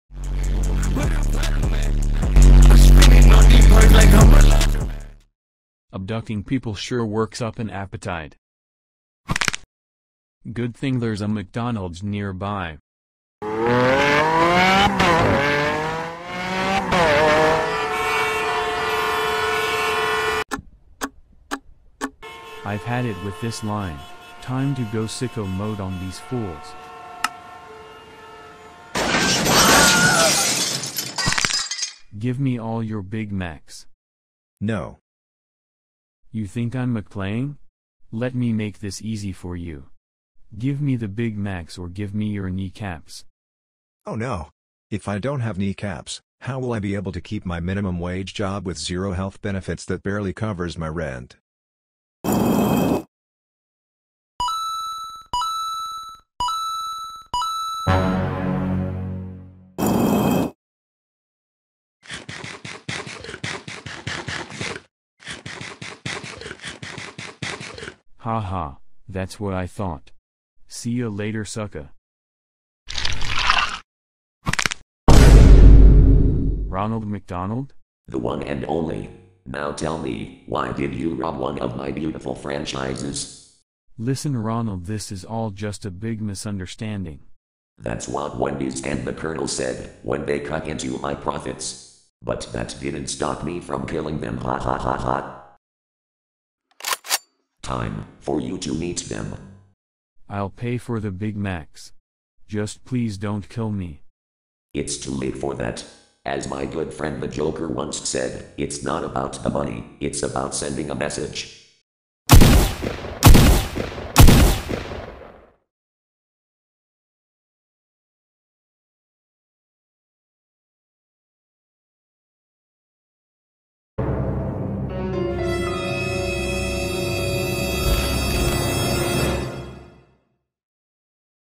Ronald Snaps
average yeat fan gets hangry and robs a mcdonald's location

if this video seems like a fever dream that's because it is

made in maya and edited in premiere pro